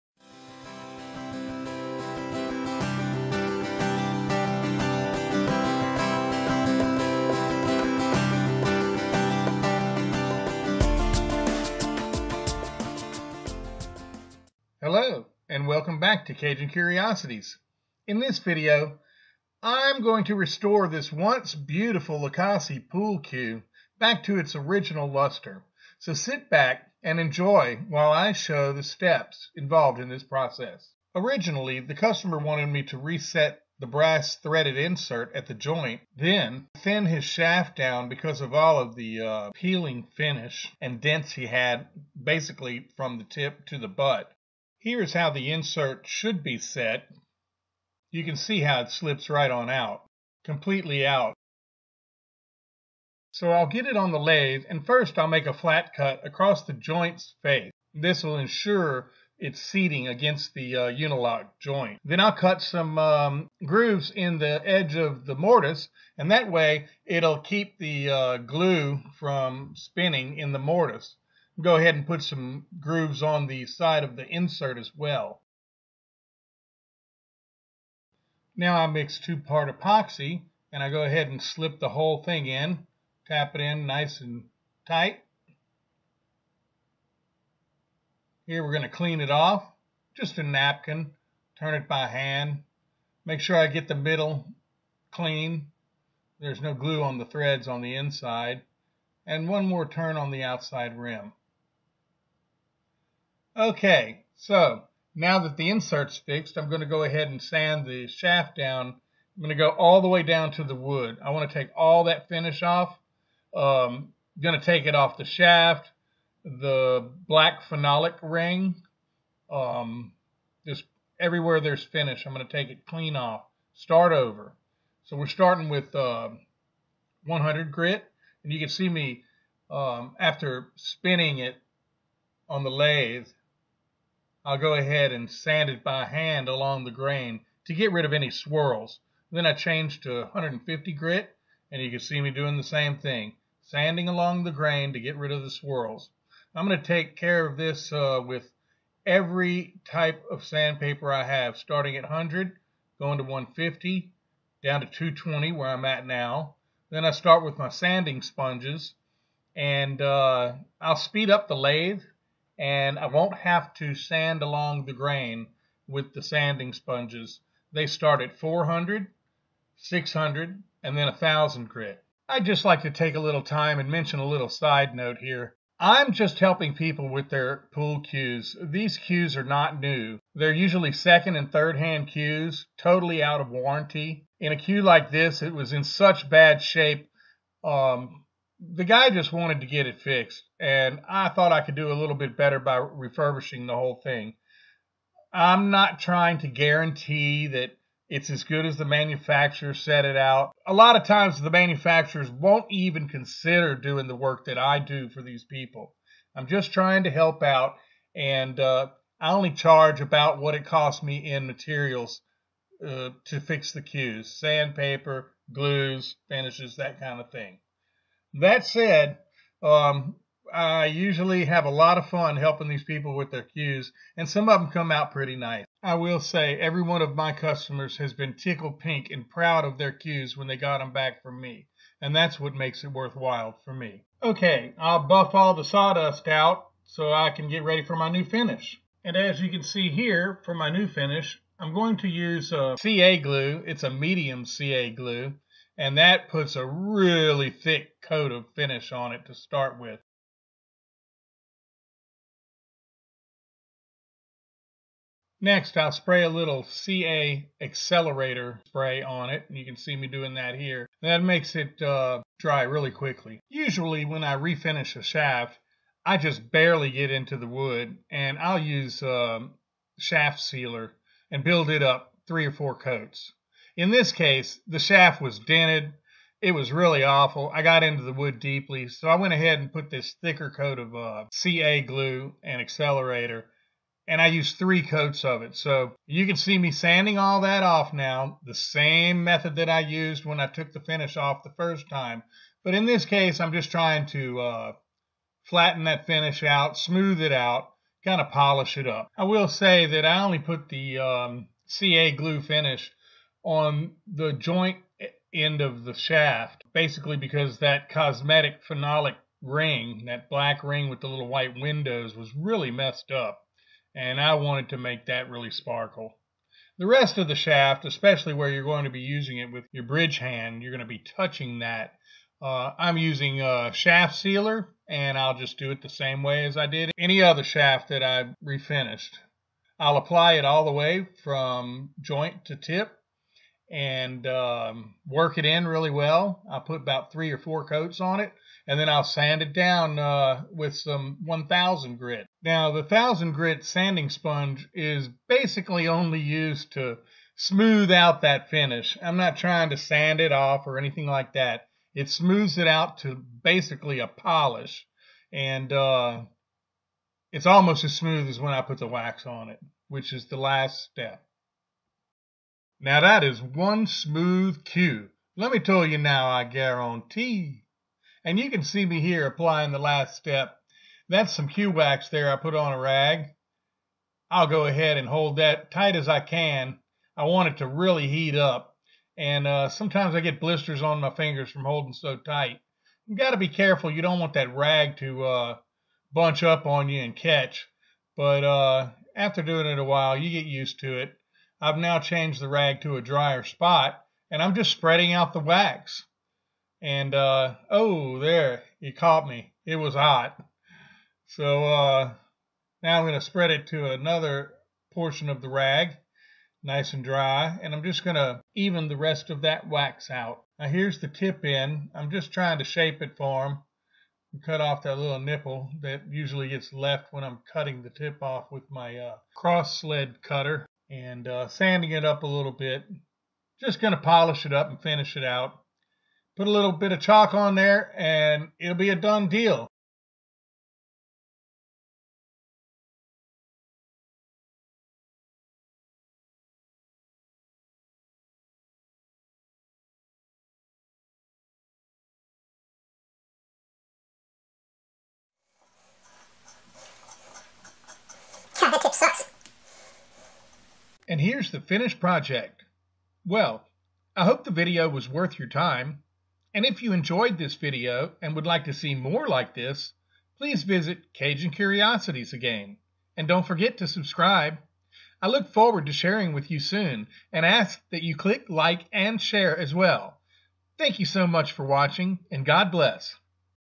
Pool Cue Repair and Complete Restoration
Repair joint insert, completely restore and refurbish Lucasi cue.